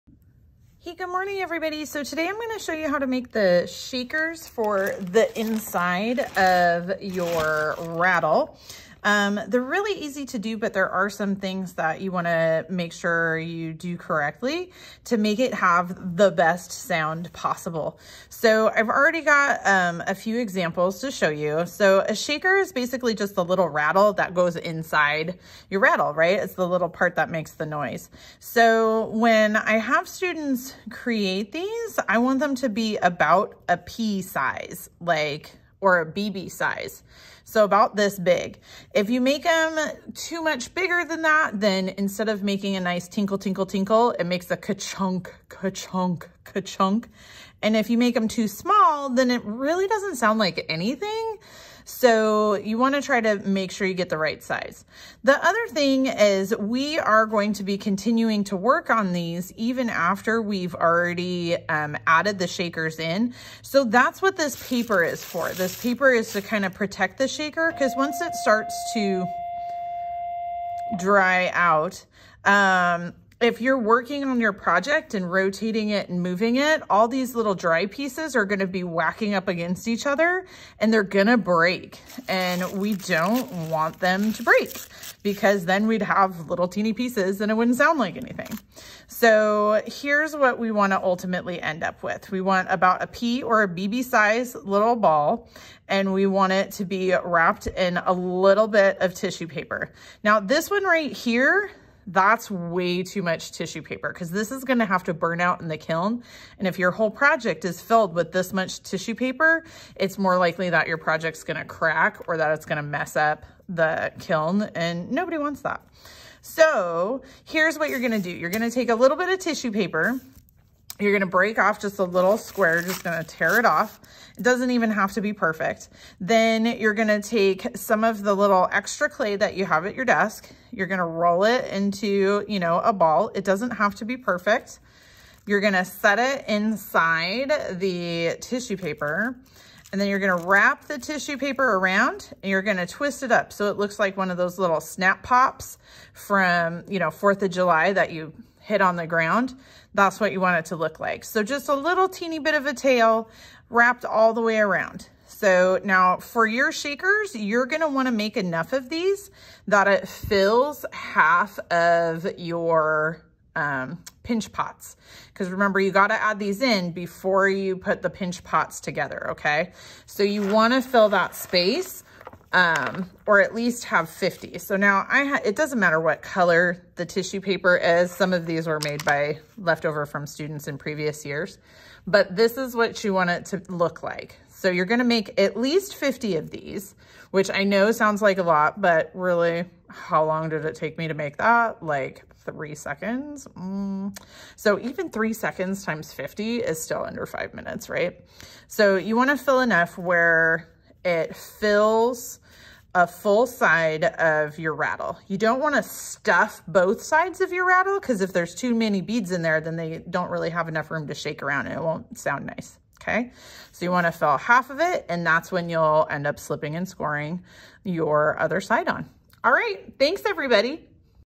Shakers for Your Clay Rattle
This demonstrates how to make the inside shakers for your clay rattle.